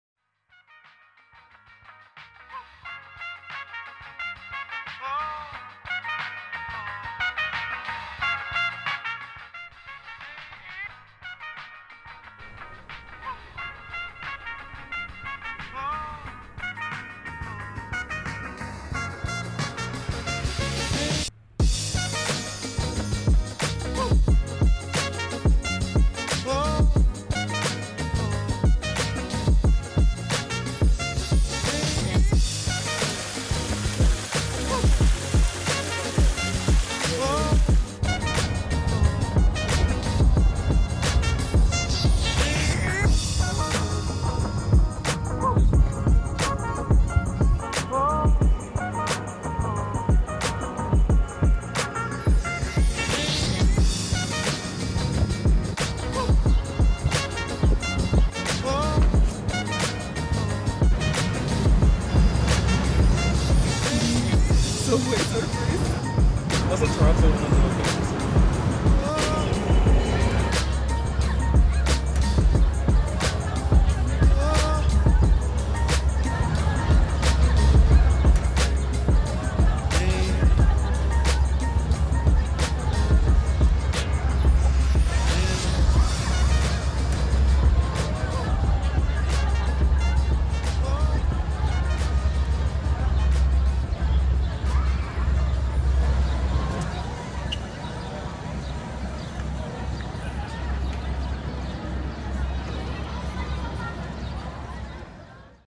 VLOG - CANADA (pt.2)
This is video is the part 2 of my experience traveling to Canada where I did a little bit of tourism in Toronto!